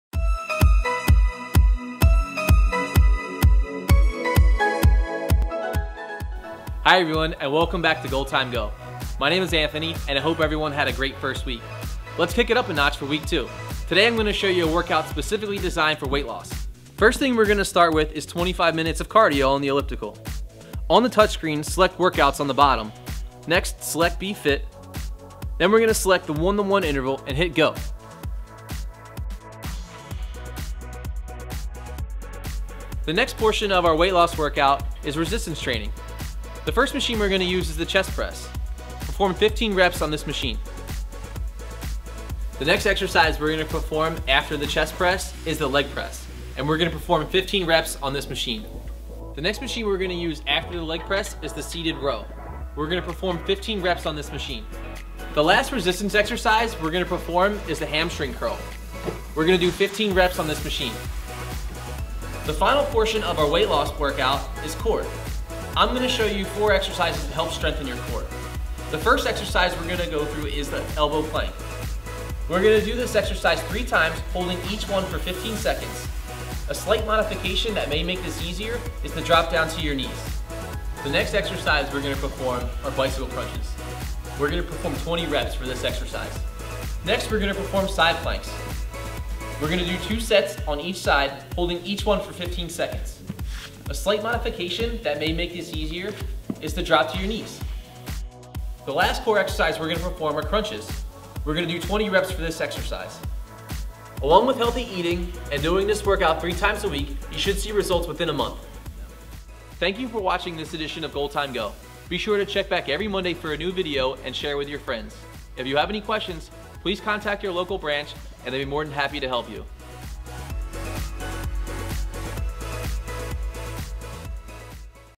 Goal Time Go Ep 3
YMCA Fitness Expert Anthony shows you the perfect workout for weight-loss!